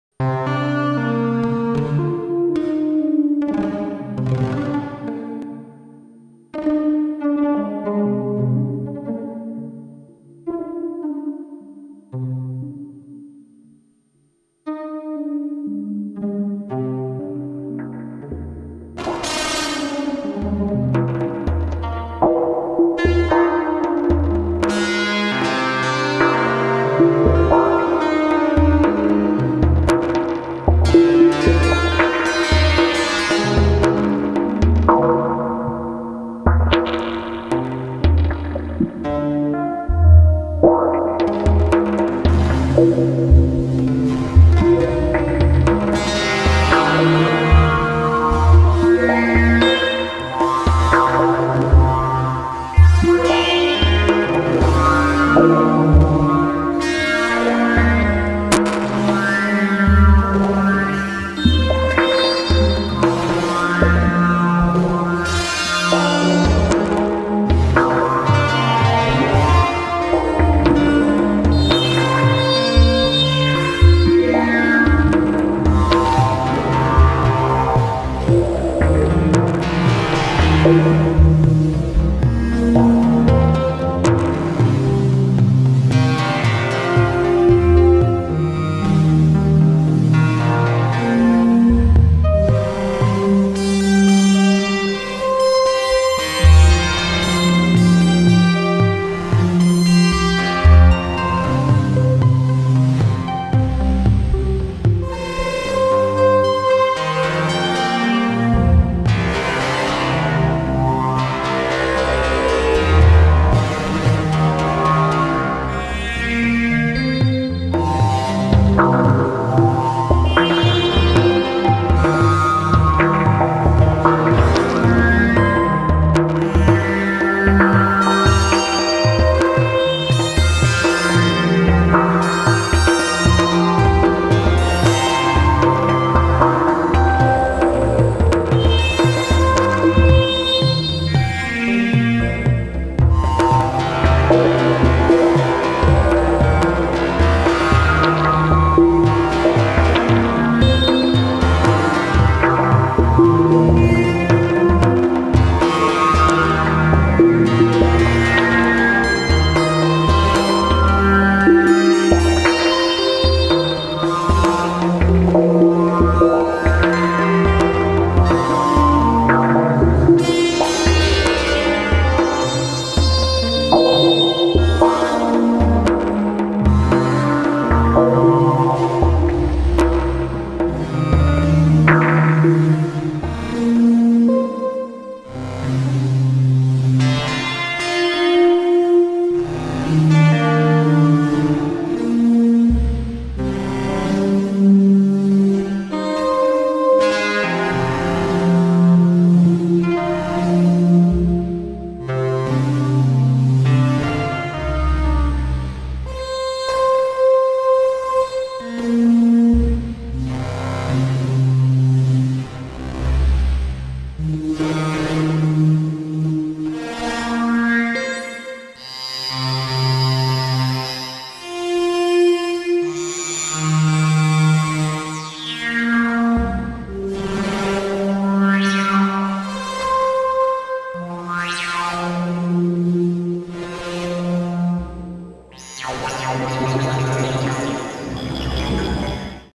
Ornament & Crime, Zadar, Qu-bit Surface, Soulsby Oscitron
Sequence from the Enigma applet on O_c with the Hemisphere firmware. Zadar is provind wacky triggers to Qu-Bit Surface, and the other voices are Soulsby Oscitron and samples on the Squid Salmple. Lots of modulation, S&H, and filters.